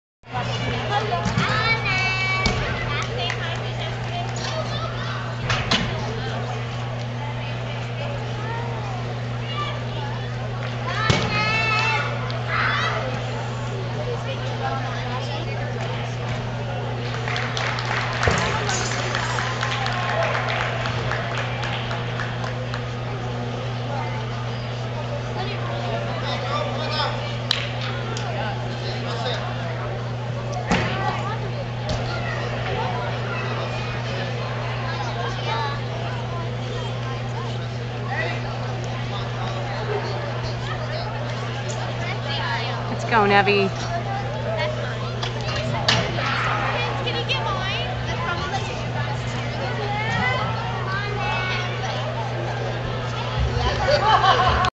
Nevaeh DeSouza Regionals 2011 level 8 Beam routine 9.575
9 year old Nevaeh DeSouza at 2011 Level 8 Reginials